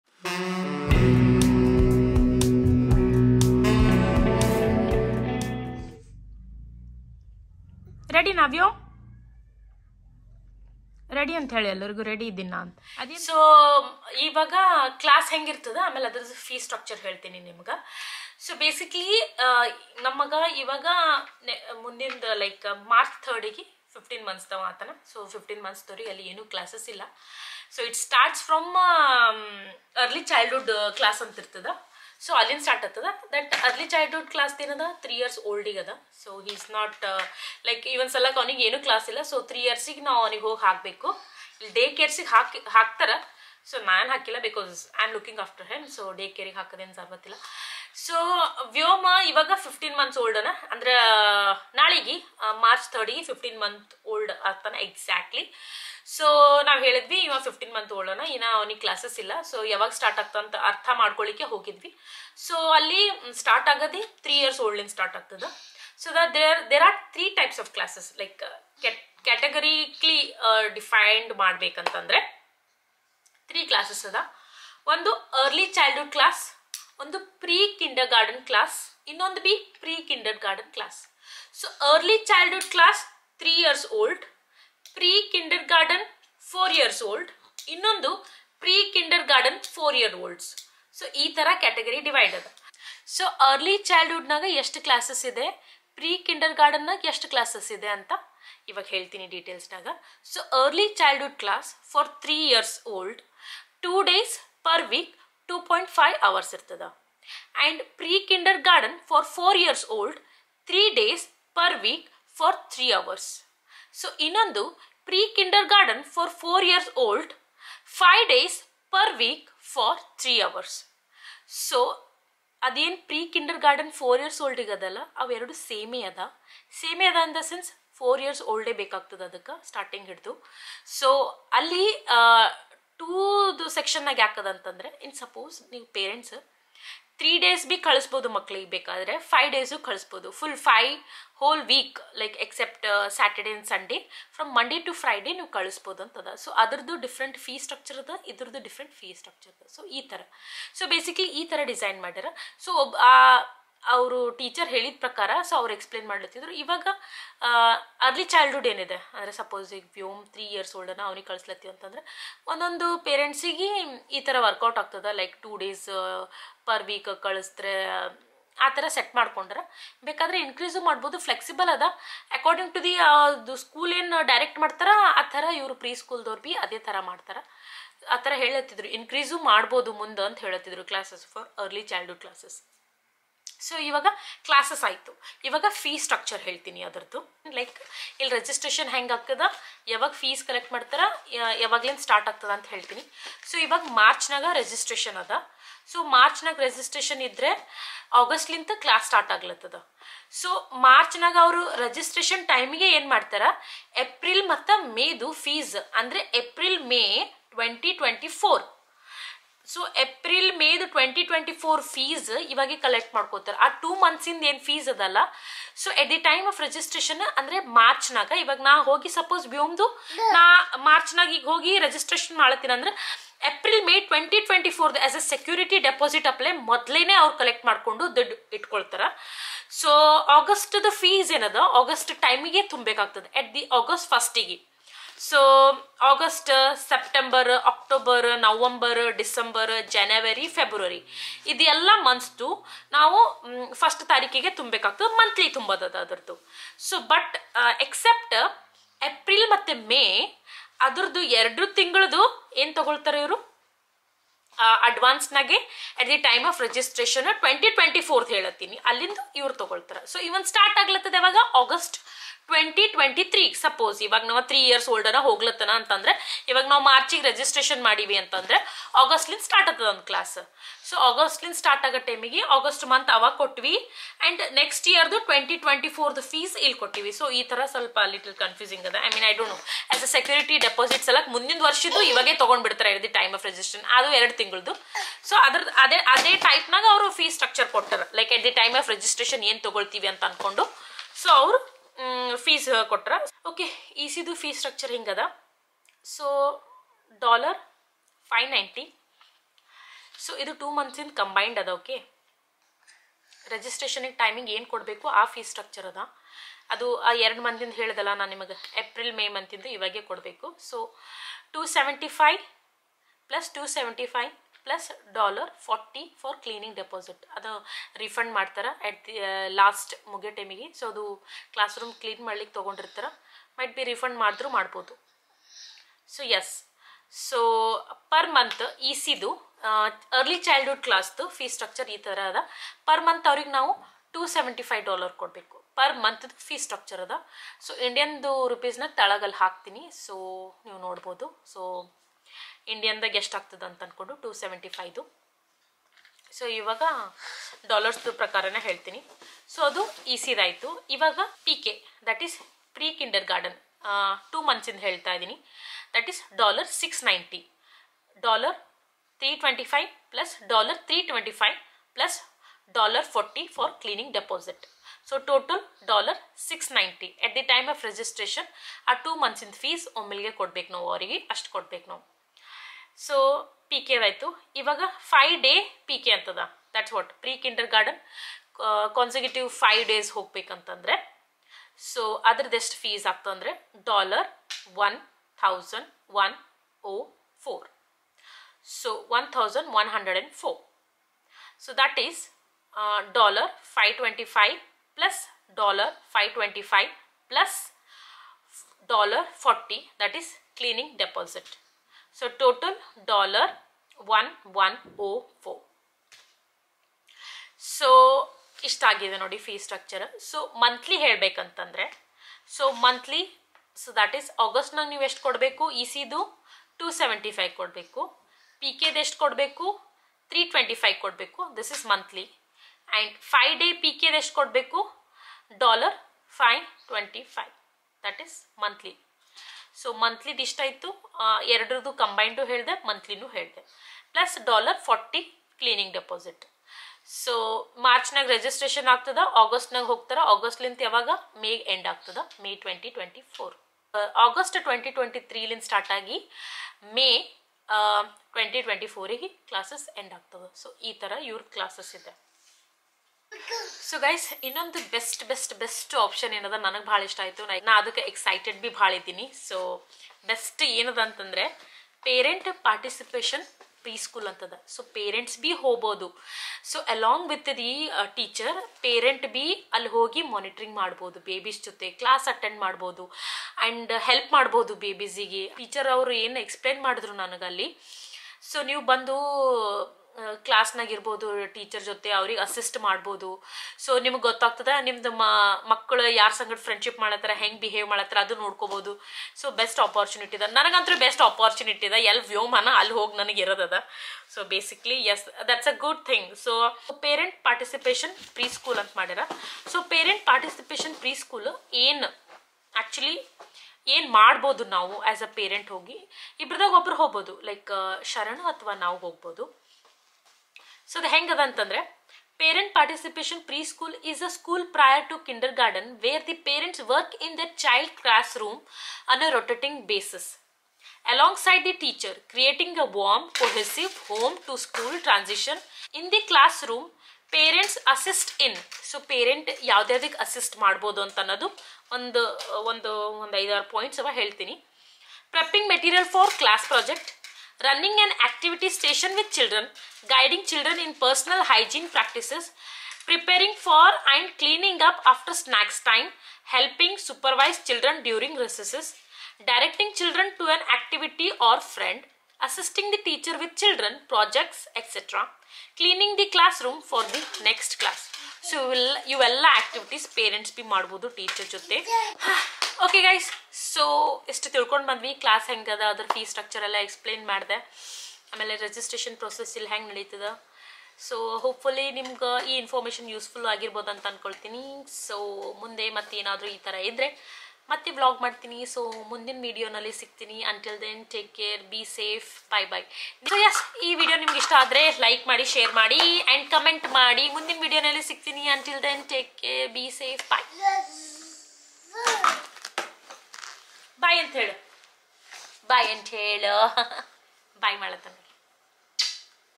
AMERICAN PRESCHOOL FEE STRUCTURE AND CLASSES | OPEN HOUSE PRESCHOOL | KANNADA VLOGS
OPEN HOUSE PRESCHOOL : This the perfect opportunity for parents to view there children classroom, meet teachers, and ask questions about their program.